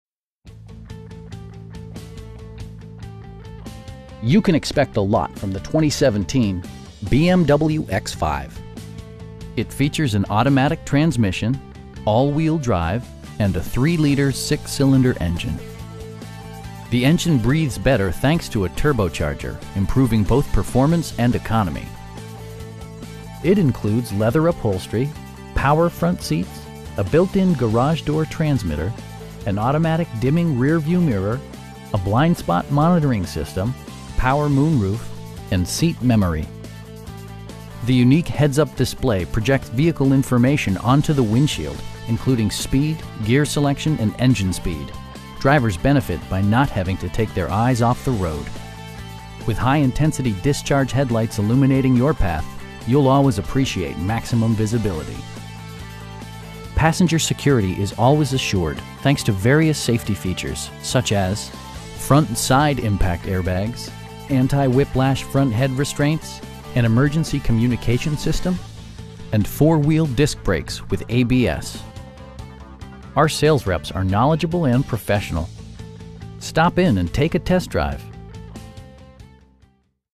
2017 BMW X5 in Charleston, SC 29407
You can expect a lot from the 2017 BMW X5 It features an automatic transmission, all-wheel drive, and a 3 liter 6 cylinder engine. The engine breathes better thanks to a turbocharger, improving both performance and economy. It includes leather upholstery, power front seats, a built-in garage door transmitter, an automatic dimming rear-view mirror, a blind spot monitoring system, power moon roof, a power rear cargo door, and seat memory. The unique heads-up display projects vehicle information onto the windshield, including speed, gear selection and engine speed. Drivers benefit by not having to take their eyes off the road. With high intensity discharge headlights illuminating your path, you'll always appreciate maximum visibility. Passenger security is always assured thanks to various safety features, such as: head curtain airbags, front side impact airbags, traction control, brake assist, anti-whiplash front head restraints, ignition disabling, an emergency communication system, and 4 wheel disc brakes with ABS. You'll never lose visibility with rain sensing wipers, which activate automatically when the drops start to fall. Our sales reps are knowledgeable and professional. They'll work with you to find the right vehicle at a price you can afford. Stop in and take a test drive!